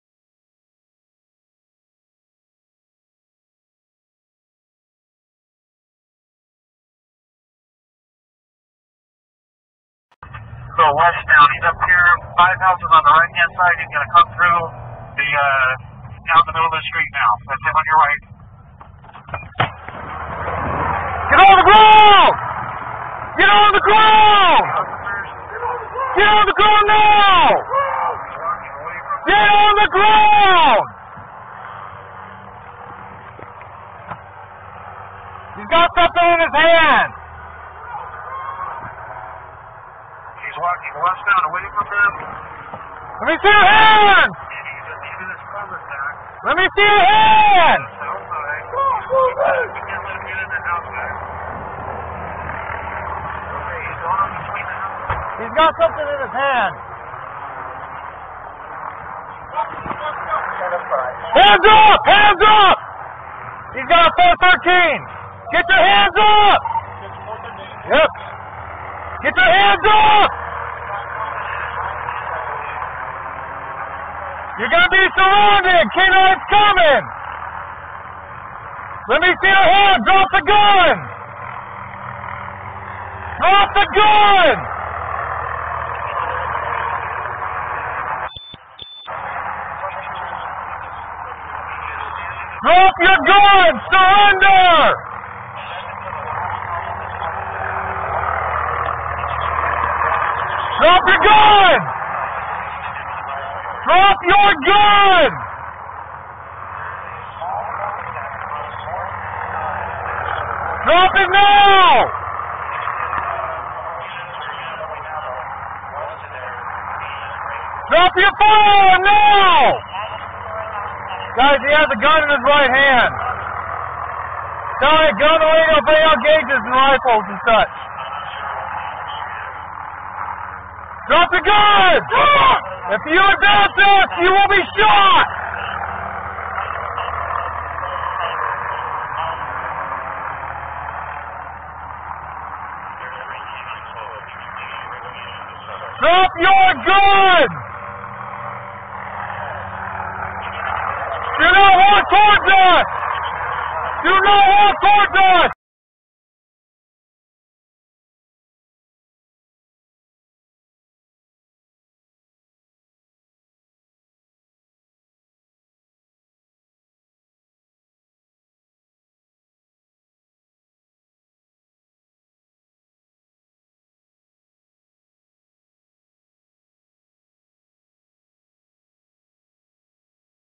LVMPD Body Cam Video of Keith Childress Jr. Shooting
This is the body camera video from one of the cops involved in the shooting of Keith Childress Jr. on New Years Eve in Las Vegas as released by the LVMPD. A second cop involved also had a bodycam that day, but didn't turn it on.

Childress was unarmed, but the officers claimed they thought a cell phone he was holding was actually a gun. They also said they were told Childress was wanted for attempted murder, although that was not true.